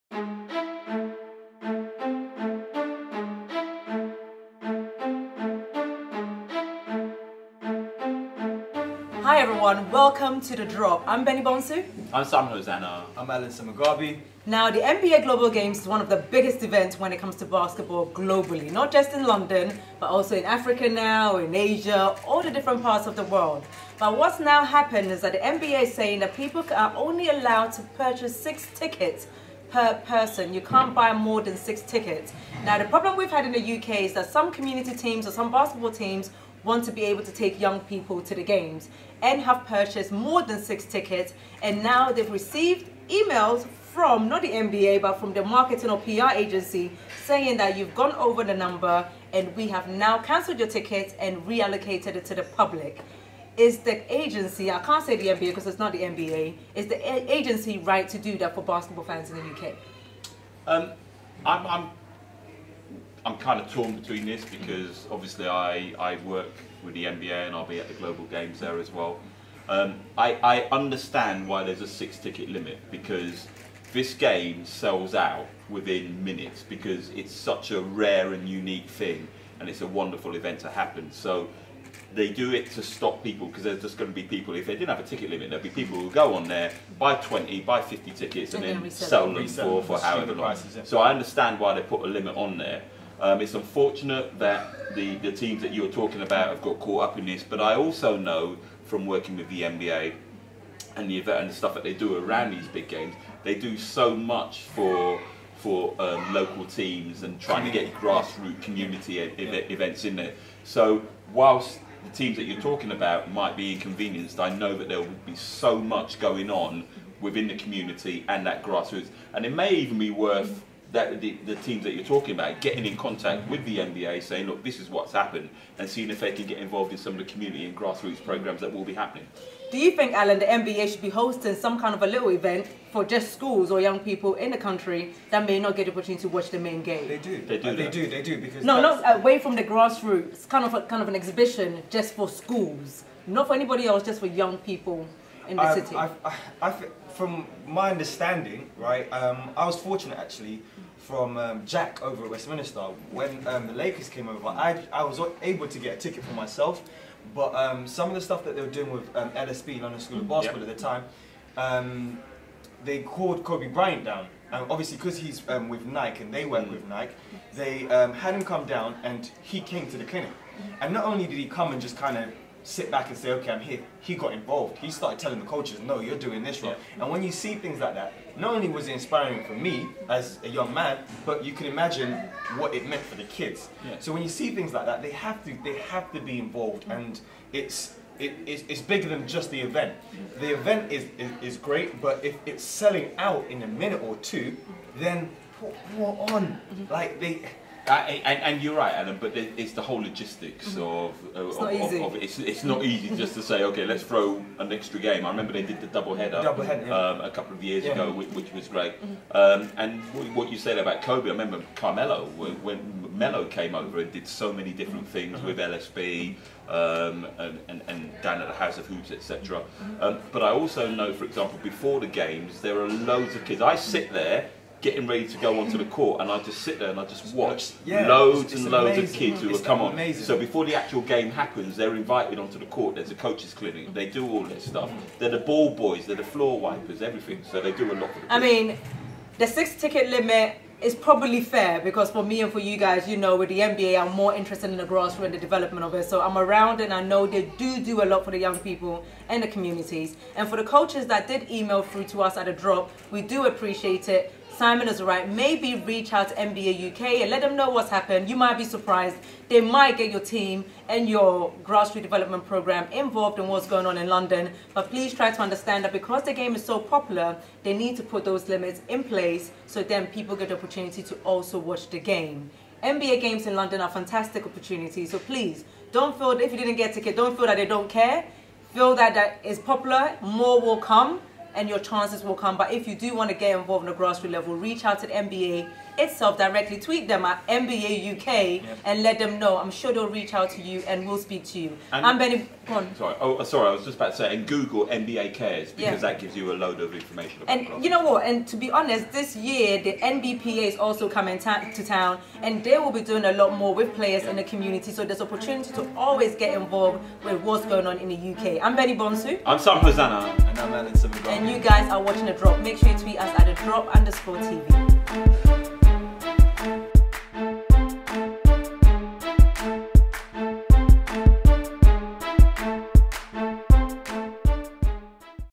Should the NBA be doing more in the community in the UK? | The Drop Basketball Show
NBA in the Community is a big deal and it is something the NBA takes seriously. Sometimes that message gets confused with the commercial side of the league. This episode, we address some of the issues fans in the UK are facing when it comes to buying tickets for the NBA Global Games.

Should the NBA be doing more in the community in the UK? Let's Talk
Production Manager: Amy Latchana
Photographer: Aysun Sen
Digital Media and Assistant Producer: Benson Mensah-Bonsu
AV: Chris Giles